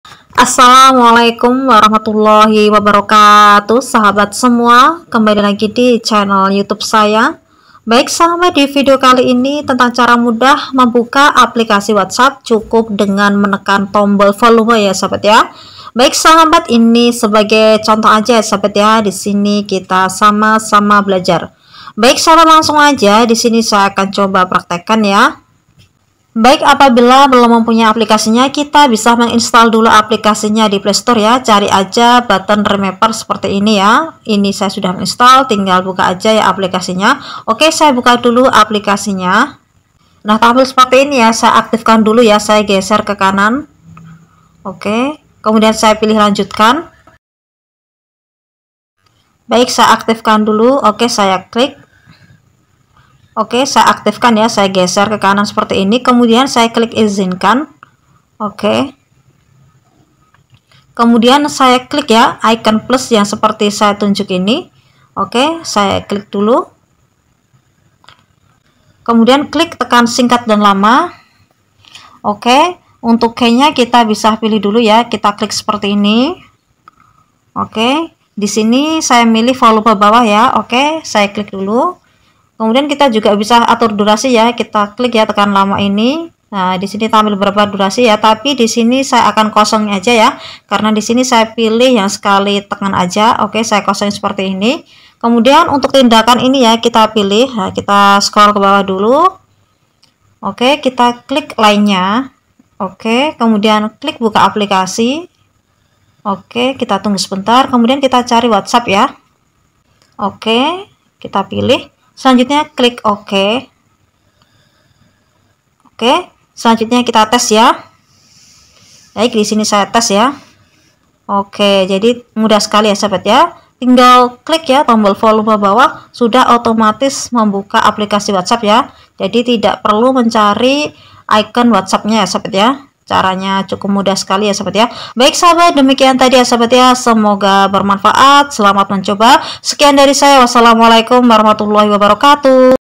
cara buka WhatsApp menggunakan tombol volume
Sahabat semua kembali lagi di channel YouTube saya, baik sahabat divideo kali ini tentang cara mudah membuka aplikasi WhatsApp cukup dengan menekan tombol volume.
Baik sahabat langsung saja saya coba praktekkan
Baik apabila belum punya aplikasinya bisa install dulu aplikasinya di play store
Download terlebih dahulu aplikasi yang bernama Buttons Remapper di Play Store. Setelah aplikasi Buttons Remapper berhasil ter-install, langsung saja buka aplikasi Buttons Remapper.

Langkah pertama yang harus dilakukan adalah meng-aktifkan layanan aksesibilitas pada masing-masing HP Android kamu. Tekan tombol Lanjutkan untuk meneruskan ke halaman Aksesibilitas.

Pada halaman Aksesibilitas, scroll ke bawah dan cari kolom Layanan yang diunduh. Kemudian aktifkan aplikasi Buttons Remapper. Bila sudah kembali ke aplikasi Buttons Remapper.

Kini aplikasi Buttons Remapper sudah menampilkan halaman utamanya. Pertama-tama aktifkan dulu fitur Layanan diaktifkan. Kemudian, tekan ikon Tambah + dan pilih menu Tekan singkat dan lama. Setelah itu, akan muncul kolom Tindakan baru. Beri checklist pada Tekan lama dan pilih waktu tekan.

 Pilih aplikasi WhatsApp dengan cara tekan panah bawah Aplikasi Terbaru. Scroll ke bawah aplikasi terbaru dan tekan Lainnya.

6. Pilih buka aplikasi dan pilih aplikasi WhatsApp. Sebenarnya, selain aplikasi WhatsApp kamu bisa memilih aplikasi lainnya.

6. Tekan tombol Ok. Selesai, kini kamu bisa ke halaman utama HP Android kamu, kemudian tekan dan tahan tombol Volume+. Secara otomatis, aplikasi WhatsApp akan terbuka.